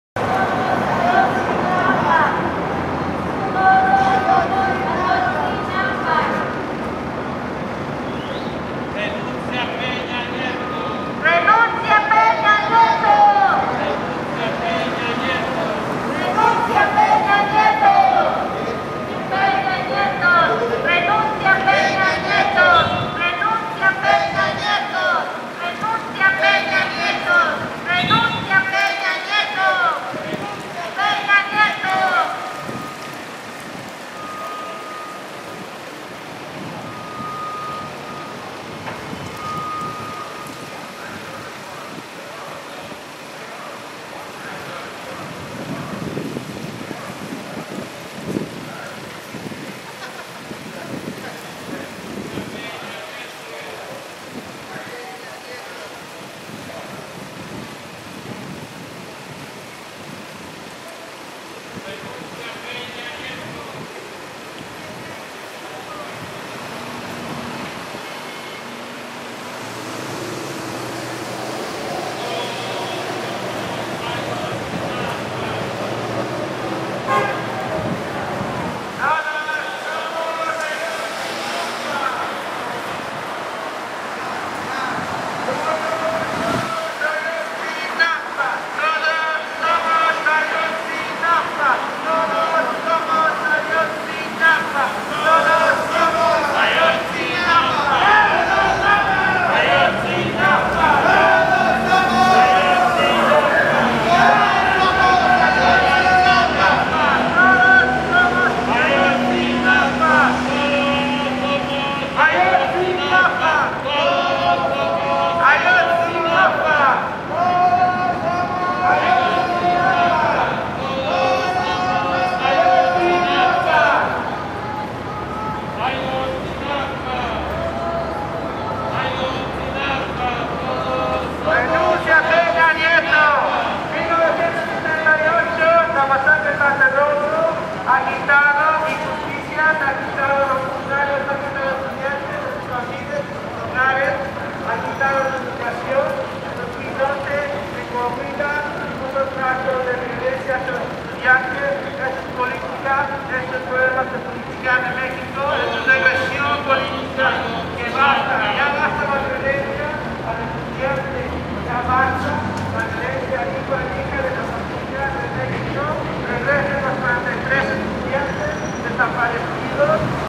10-Ayotzinapa Protest - Mexican Consulate in San Antonio, TX
Protest demonstration in front of the Mexican Consulate in San Antonio, Texas. November 11, 2014. Regards the disappearance of 43 male student teachers attacked by municipal police and then handed over to the Guerreros Unidos drug gang on September 26, 2014 in Iguala, Guerrero, Mexico. The students attended the Escuela Normal Rural Raúl Isidro Burgos in Ayotzinapa, about 70 miles north of Acapulco.